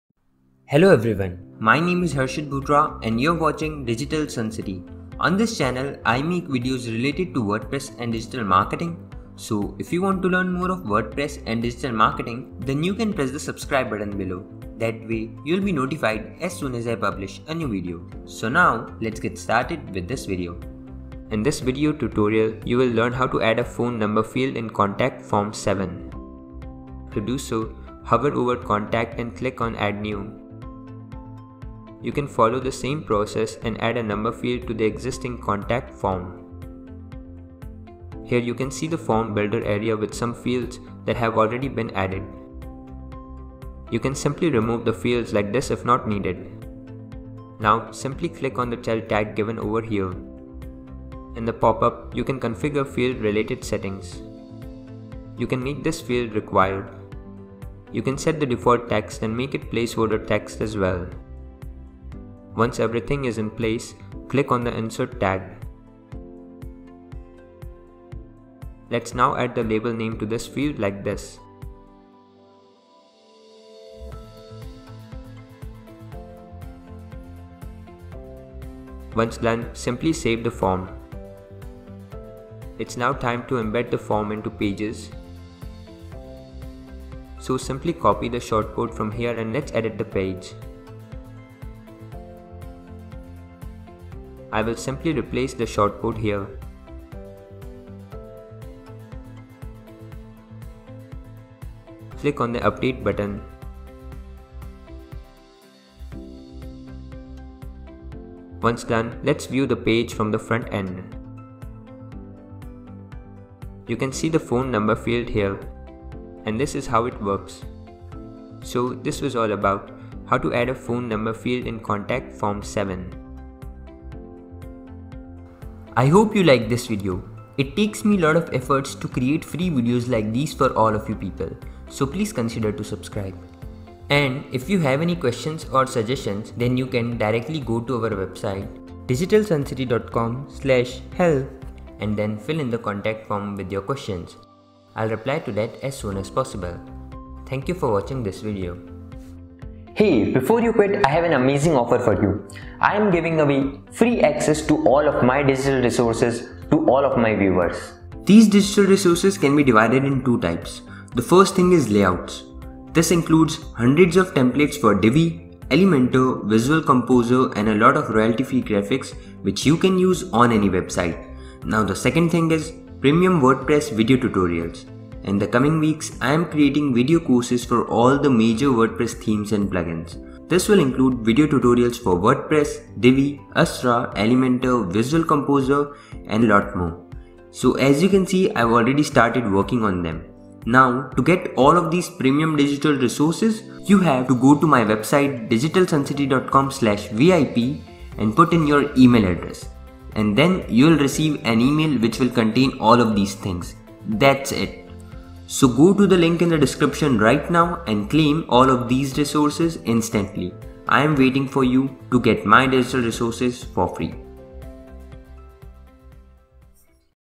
How To Add Phone Number Field In Contact Form 7 | WordPress Tutorial
Do you want to add a phone number field in contact form 7?🤔

►In this video, you’ll learn how to add a phone number field to your contact form 7 in WordPress. This is a great way to give your website visitors an easy way to contact you, and it can also be useful for gathering information for your business.

►Whether you are a blogger, a business owner, or just someone who wants to create a professional website, this tutorial will help you take your website to the next level.